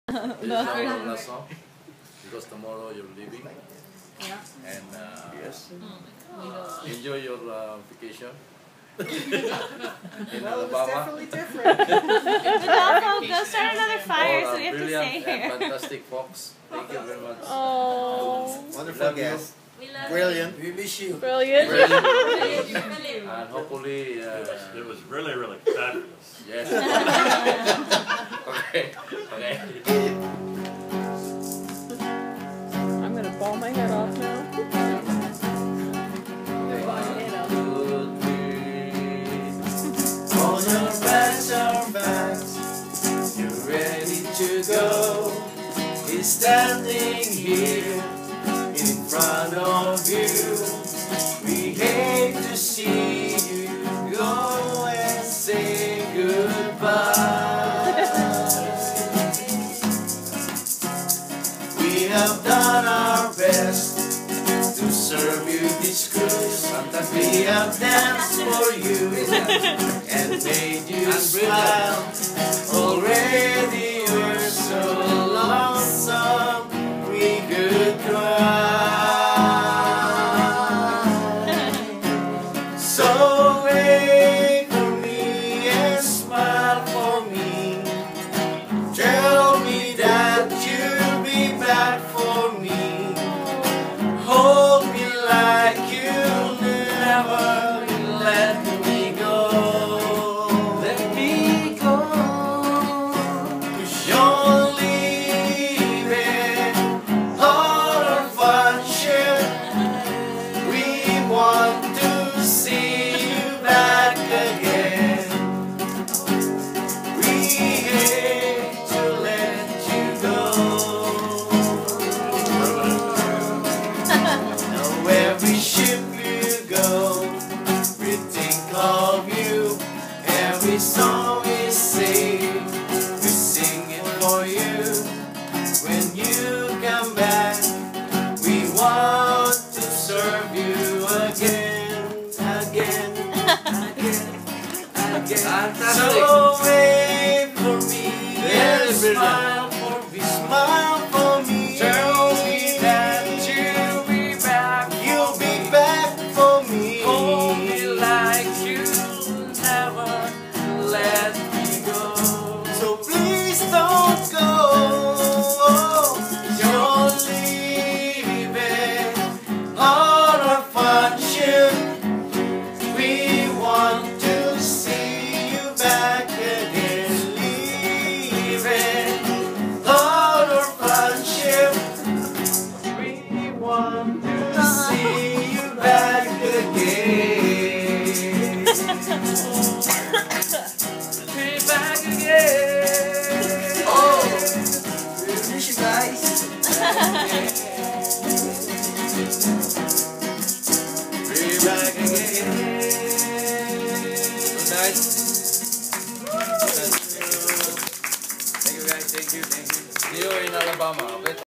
Carnival Triumph 2013 - Lifeboat 11
This was the last night we were on board the Triumph and also their last song to us.